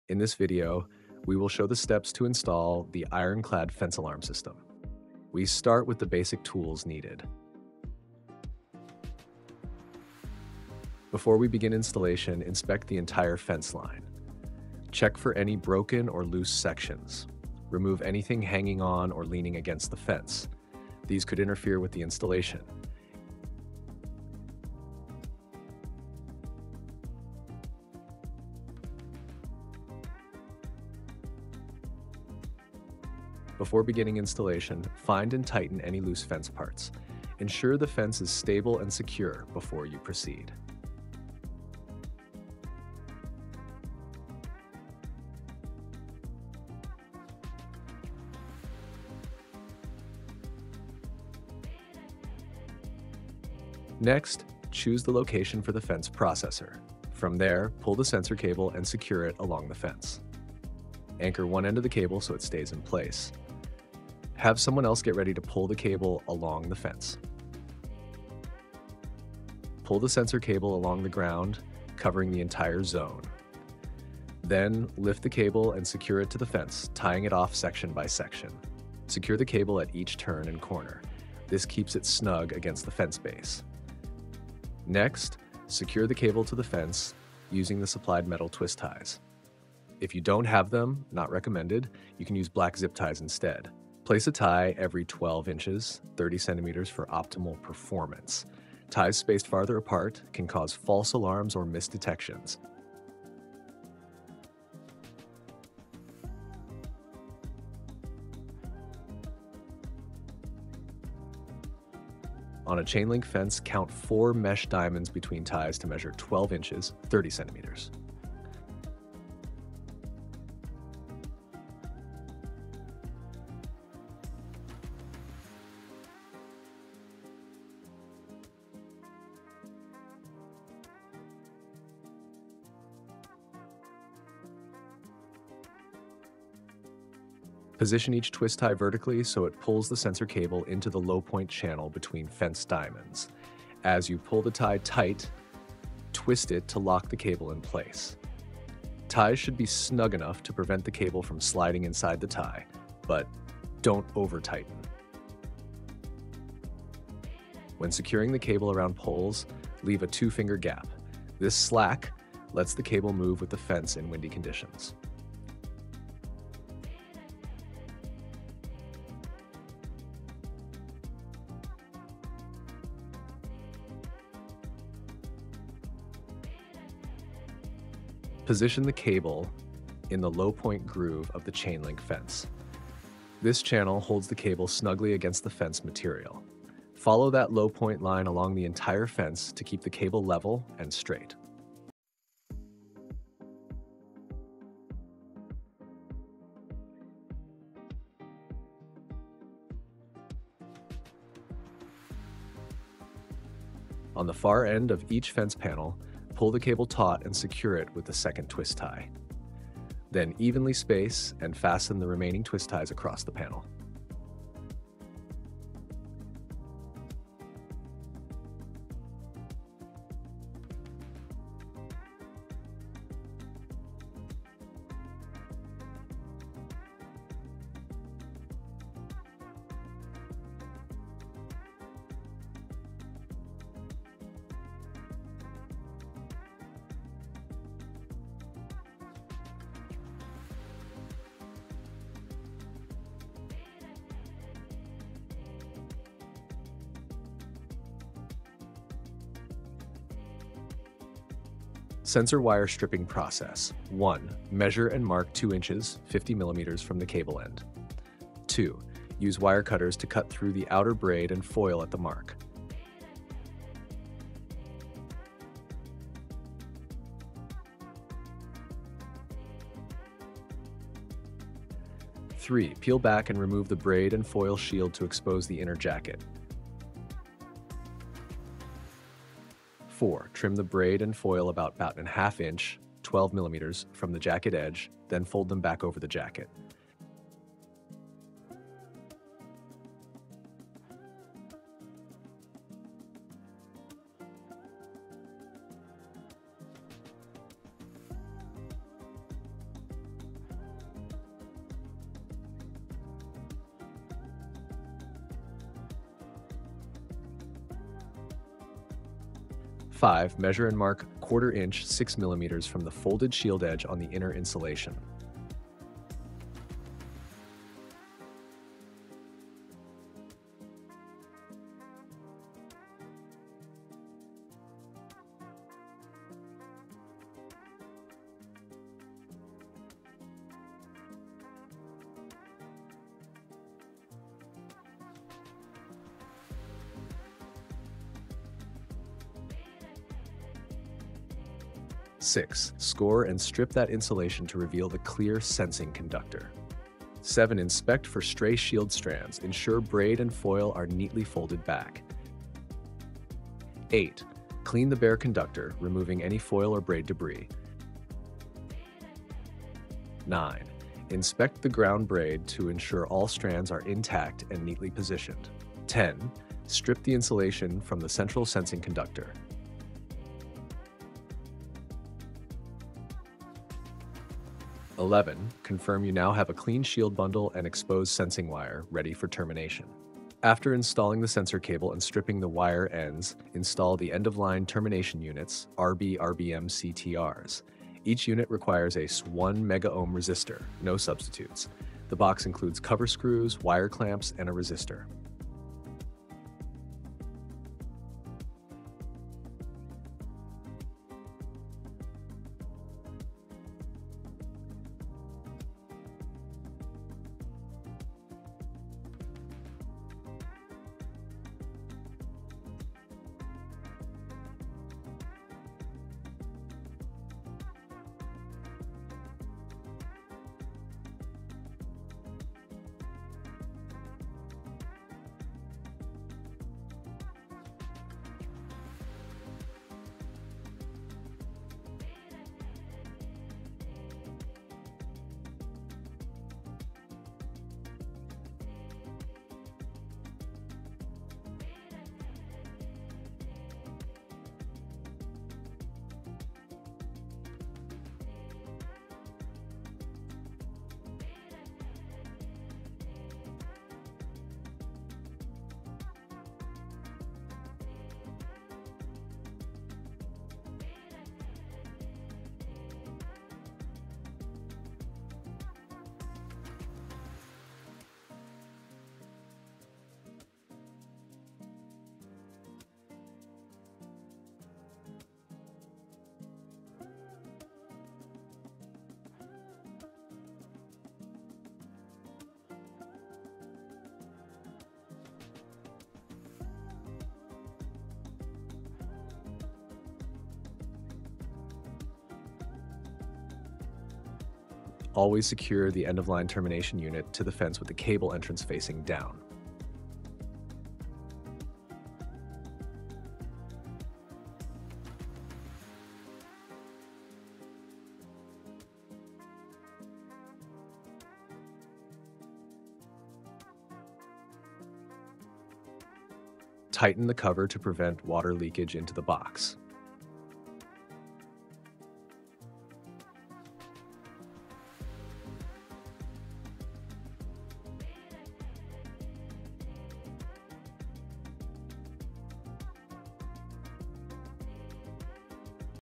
IRONCLAD Fence Alarm System - Sensor Cable Installation
Boost the security of your perimeter with a flawless sensor cable installation for the IRONCLAD Fence Alarm System. In this step‑by‑step guide, you’ll learn how to:

- Prepare and strip the sensor cable
- Mount the Delta Blocks along the fence line
- Terminate the cable with the RB‑RBMCTR end‑of‑line unit
- Secure and orient the termination box for optimal weather protection
- Verify proper resistor seating and ground braid integrity

Whether you’re a professional installer or a hands‑on facility manager, this video walks you through every detail to ensure your fence intrusion detection is both reliable and maintenance‑free.

🔥 Watch, Learn, and Safeguard Your Site Today!🔥

Chapters:
0:00 Intro
0:05 Tools needed
0:14 Fence Inspection
0:56 Location of the processor and start the installation
1:26 How to tie the cable to the fence
2:38 Spacing around poles
4:10 How to strip the sensor wire
6:20 How to install the end-of-line termination unit